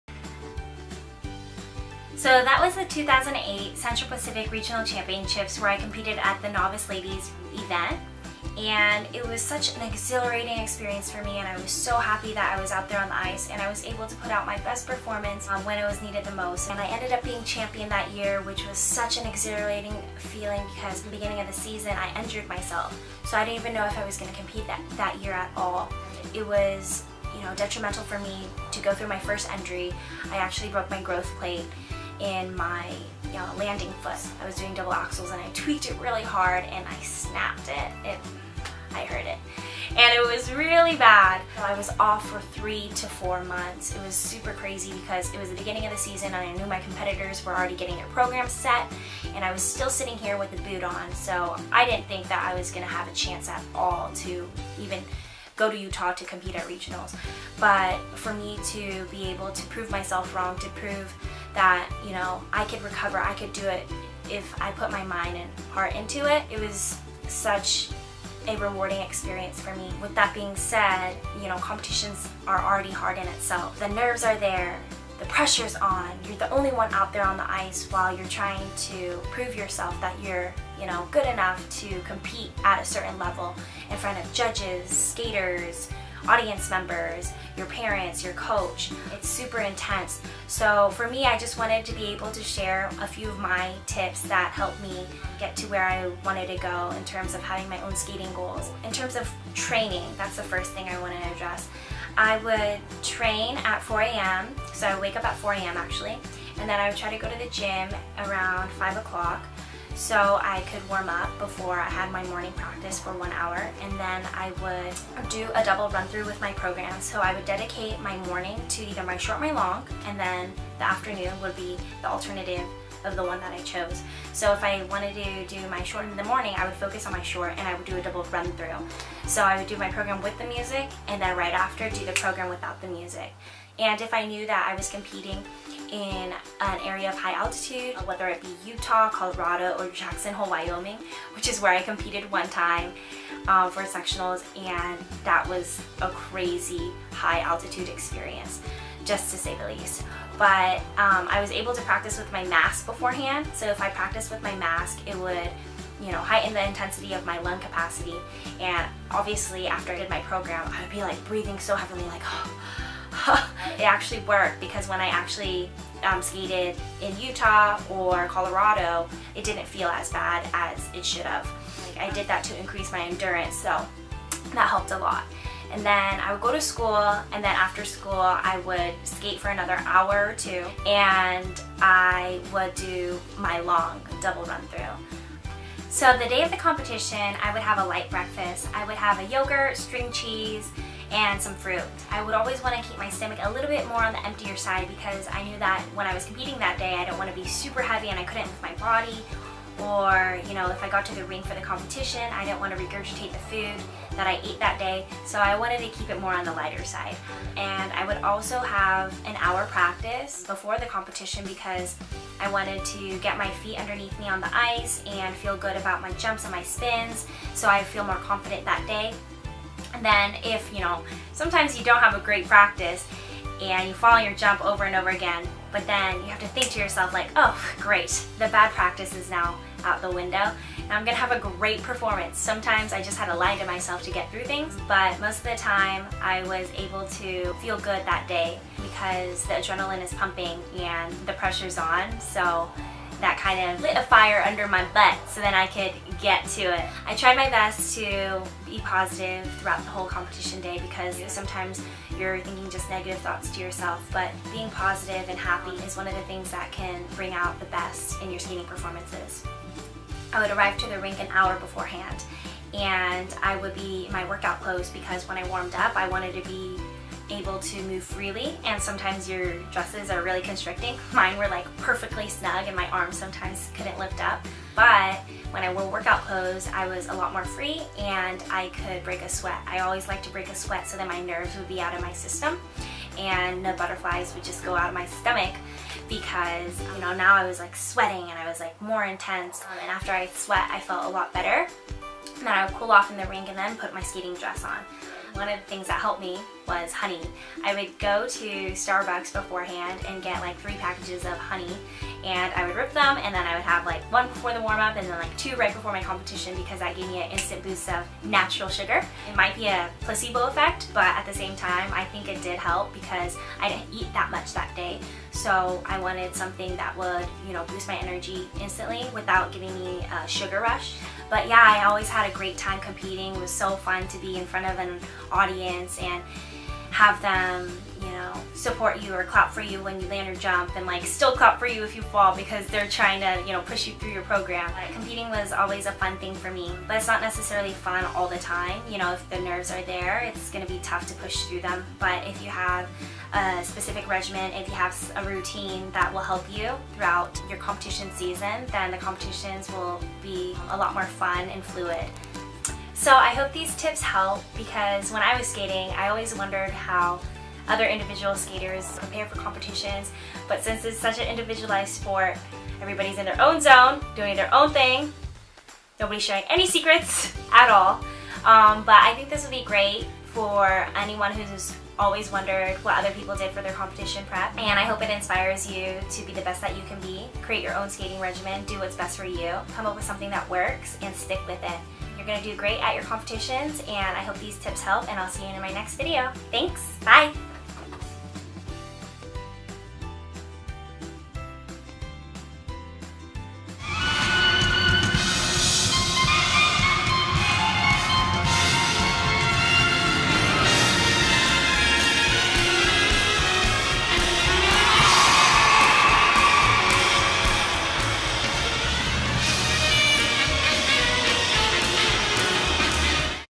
HOW TO PREPARE FOR FIGURE SKATING COMPETITIONS | Coach Michelle Hong
I wanted to share my personal tips on my journey to becoming Central Pacific Regional Championships in hopes that I can help those in the skating community who are currently preparing for upcoming competitions. Every skater is different so find out what works for you and stick to it. With hard work, passion, and commitment, you can reach your skating goals too! Good luck on your journey and remember to stay positive during times of doubt and frustration.

Always,
Coach Michelle Hong